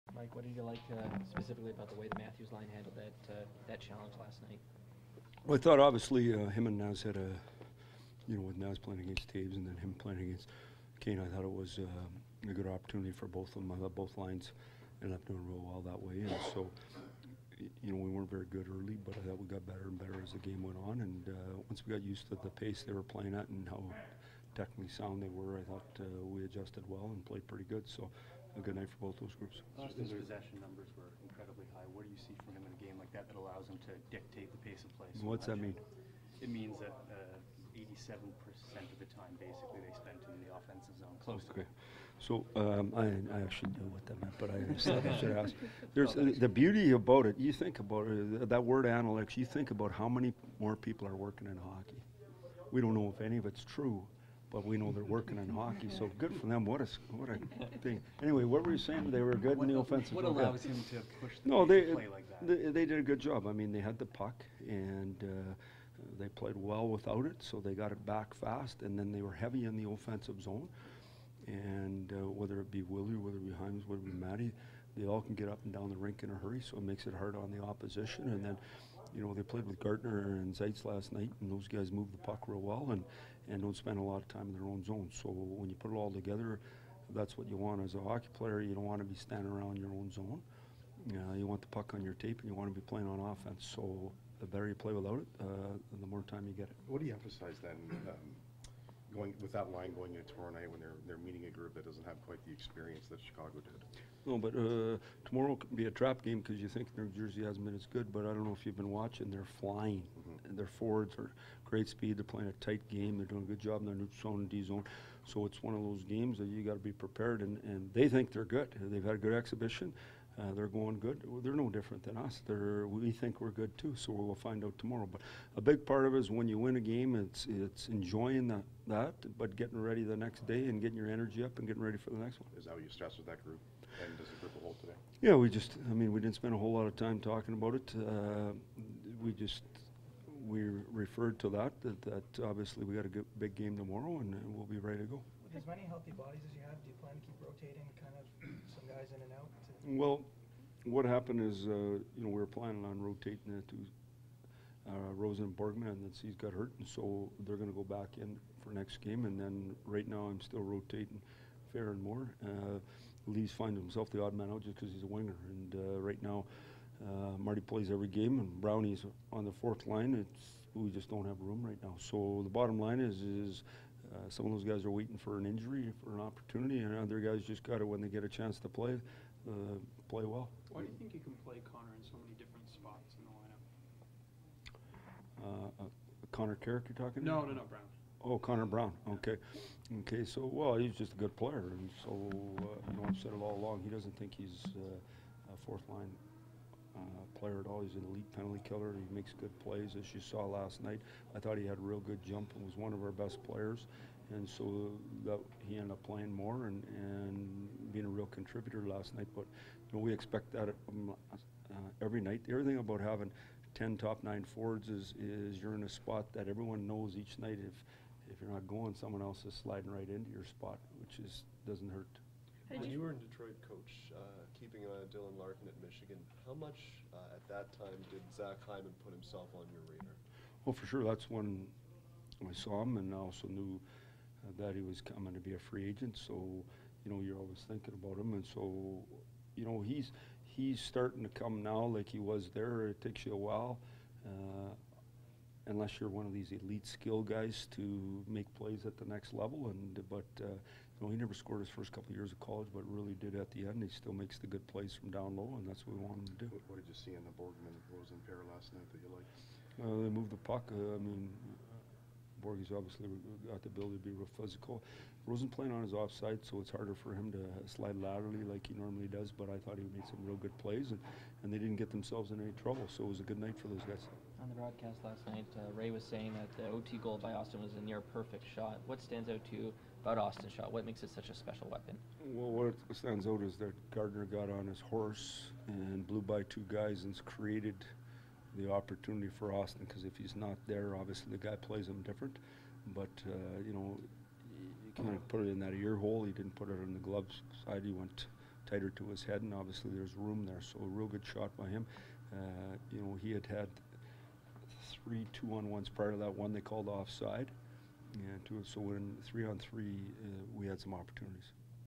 Maple Leafs Practice: Mike Babcock - October 10, 2017
Mike Babcock speaks with members of the media following Tuesday's practice.